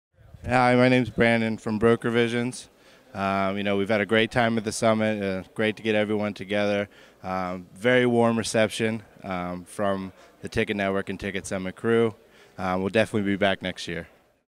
Ticket Summit 2013 Vegas -- Brandon Dunn (Testimonial)
Brandon Dunn, Broker Visions, gives a testimonial about Ticket Summit 2013 at Bellagio Resort Hotel and Casino in Las Vegas, Nevada. Ticket Summit is the world's leading ticket conference and trade show for live entertainment professionals. for more information about our year-round events.